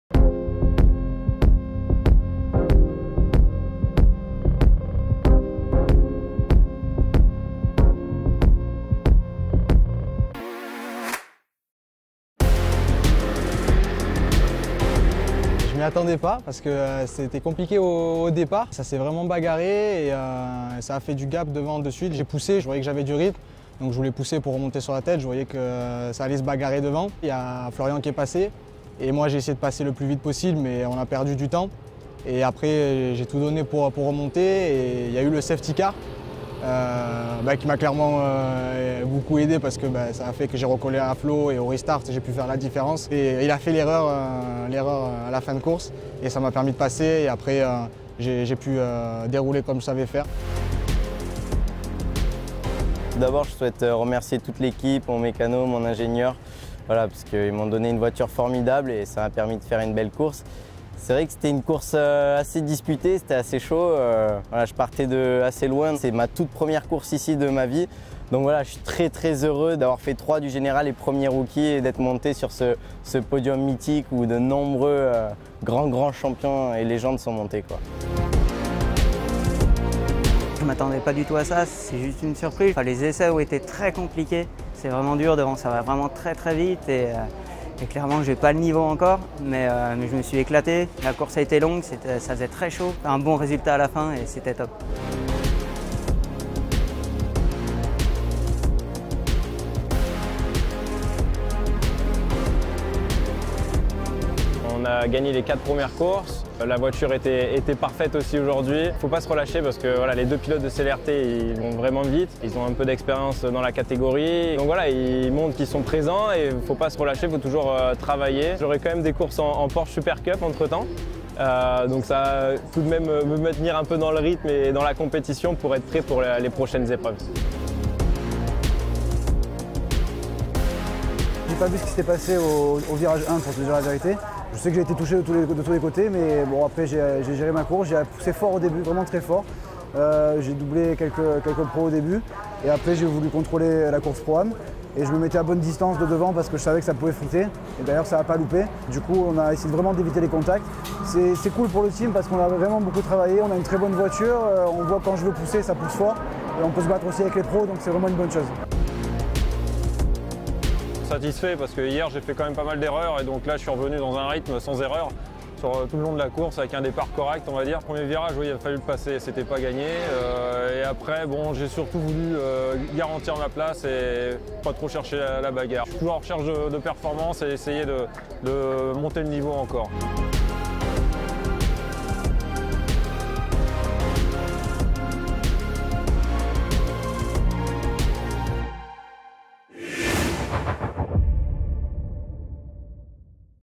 PCCF​​ - Highlights Manche 4 - Monza - Porsche Carrera Cup France
Prêts à revivre les deux courses passionnantes au cœur du temple de la vitesse ?